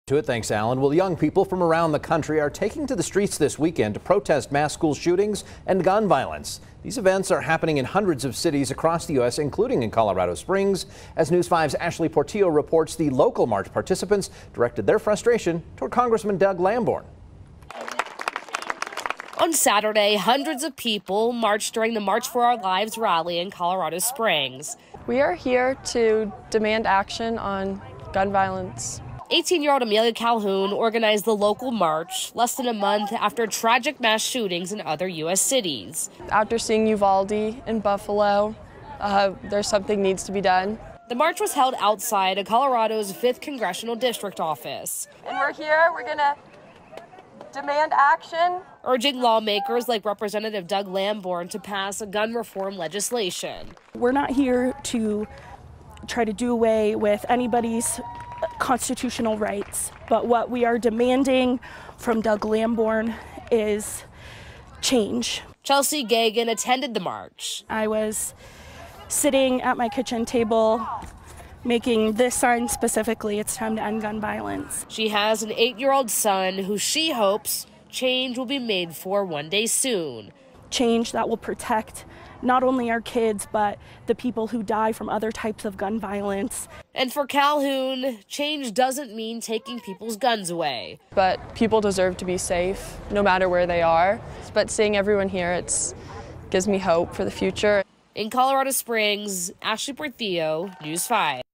Colorado Springs protest over mass shootings and gun violence
Young people from around the country are taking to the streets this weekend to protest mass school shootings and gun violence. These events are happening in hundreds of cities across the United States, including Colorado Springs.